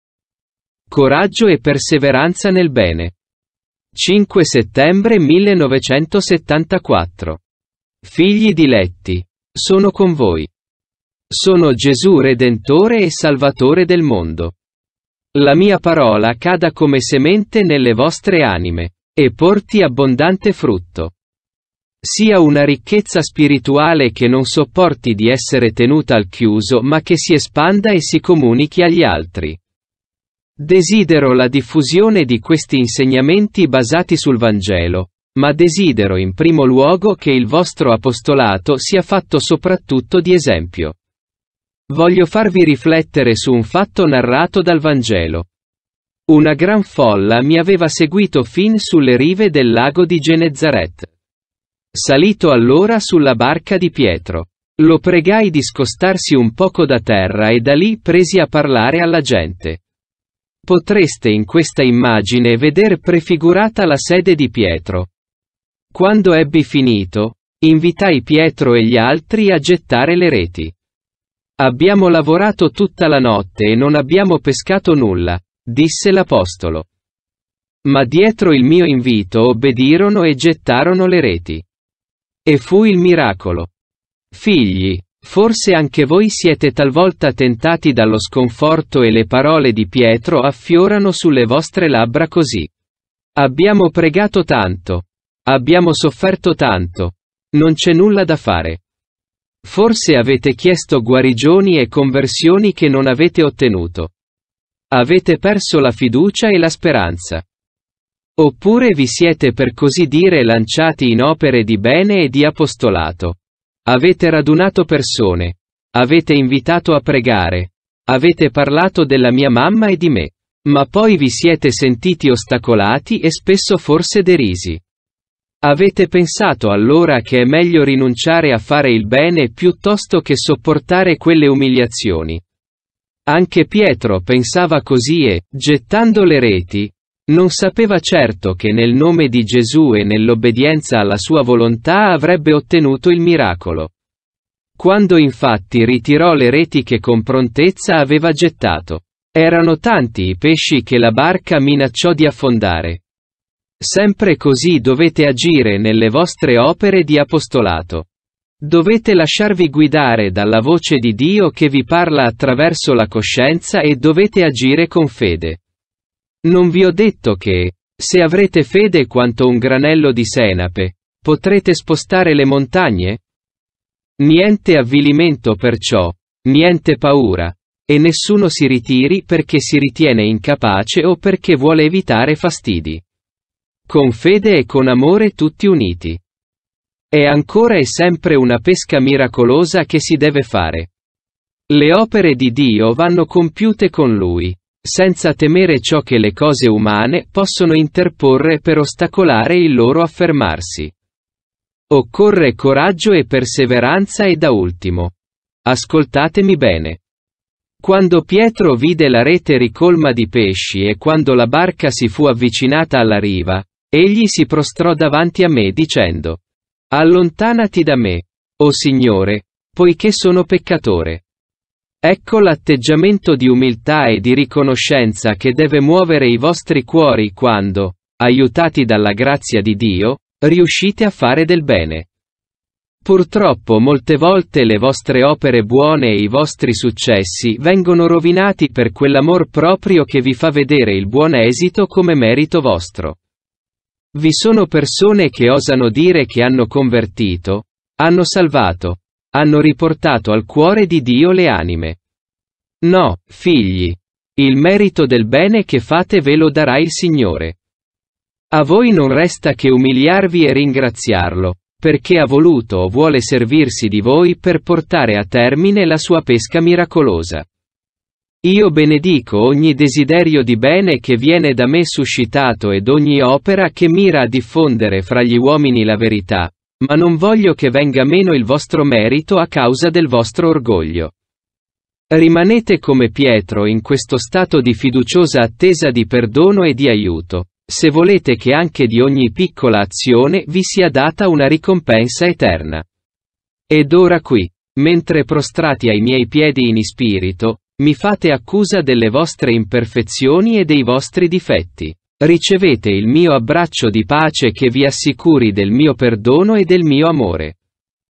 Coraggio e perseveranza nel bene
GESÙ NOSTRO MAESTRO 5. Mamma Carmela 5 settembre 1974.

Figli diletti, sono con voi. Sono Gesù Redentore e Salvatore del mondo. La mia Parola cada come semente nelle vostre anime, e porti abbondante frutto. Sia una ricchezza spirituale che non sopporti di essere tenuta al chiuso ma che si espanda e si comunichi agli altri.
Desidero la diffusione di questi insegnamenti basati sul Vangelo, ma desidero in primo luogo che il vostro apostolato sia fatto soprattutto di esempio.
Voglio farvi riflettere su un fatto narrato dal Vangelo. Una gran folla mi aveva seguito fin sulle rive del lago di Genezaret. Salito allora sulla barca di Pietro, lo pregai di scostarsi un poco da terra e da lì presi a parlare alla gente. Potreste in questa immagine veder prefigurata la sede di Pietro.
Quando ebbi finito, invitai Pietro e gli altri a gettare le reti. «Abbiamo lavorato tutta la notte e non abbiamo pescato nulla», disse l'apostolo. Ma dietro il mio invito obbedirono e gettarono le reti. E fu il miracolo.
Figli, forse anche voi siete talvolta tentati dallo sconforto e le parole di Pietro affiorano sulle vostre labbra così: «Abbiamo pregato tanto, abbiamo sofferto tanto, non c'è nulla da fare».
Forse avete chiesto guarigioni e conversioni che non avete ottenuto. Avete perso la fiducia e la speranza. Oppure vi siete per così dire lanciati in opere di bene e di apostolato: avete radunato persone, avete invitato a pregare, avete parlato della mia Mamma e di Me, ma poi vi siete sentiti ostacolati e spesso forse derisi. Avete pensato allora che è meglio rinunciare a fare il bene piuttosto che sopportare quelle umiliazioni.
Anche Pietro pensava così e, gettando le reti, non sapeva certo che nel nome di Gesù e nell'obbedienza alla sua volontà avrebbe ottenuto il miracolo. Quando infatti ritirò le reti che con prontezza aveva gettato, erano tanti i pesci che la barca minacciò di affondare.
Sempre così dovete agire nelle vostre opere di apostolato. Dovete lasciarvi guidare dalla voce di Dio che vi parla attraverso la coscienza e dovete agire con fede.
Non vi ho detto che, se avrete fede quanto un granello di senape, potrete spostare le montagne? Niente avvilimento perciò, niente paura; e nessuno si ritiri perché si ritiene incapace o perché vuole evitare fastidi.
Con fede e con amore tutti uniti. È ancora e sempre una pesca miracolosa che si deve fare. Le opere di Dio vanno compiute con Lui, senza temere ciò che le cose umane possono interporre per ostacolare il loro affermarsi. Occorre coraggio e perseveranza e da ultimo... ascoltatemi bene.
Quando Pietro vide la rete ricolma di pesci e quando la barca si fu avvicinata alla riva, Egli si prostrò davanti a me dicendo: «Allontanati da Me, o Signore, poiché sono peccatore».
Ecco l'atteggiamento di umiltà e di riconoscenza che deve muovere i vostri cuori quando, aiutati dalla grazia di Dio, riuscite a fare del bene. Purtroppo molte volte le vostre opere buone e i vostri successi vengono rovinati per quell'amor proprio che vi fa vedere il buon esito come merito vostro. Vi sono persone che osano dire che hanno convertito, hanno salvato, hanno riportato al Cuore di Dio le anime.
No, figli, il merito del bene che fate ve lo darà il Signore. A voi non resta che umiliarvi e ringraziarlo, perché ha voluto o vuole servirsi di voi per portare a termine la sua pesca miracolosa.
Io benedico ogni desiderio di bene che viene da Me suscitato ed ogni opera che mira a diffondere fra gli uomini la verità; ma non voglio che venga meno il vostro merito a causa del vostro orgoglio.
Rimanete come Pietro in questo stato di fiduciosa attesa di perdono e di aiuto, se volete che anche di ogni piccola azione vi sia data una ricompensa eterna.
Ed ora qui, mentre prostrati ai miei piedi in ispirito mi fate accusa delle vostre imperfezioni e dei vostri difetti, ricevete il mio abbraccio di pace che vi assicuri del mio perdono e del mio amore.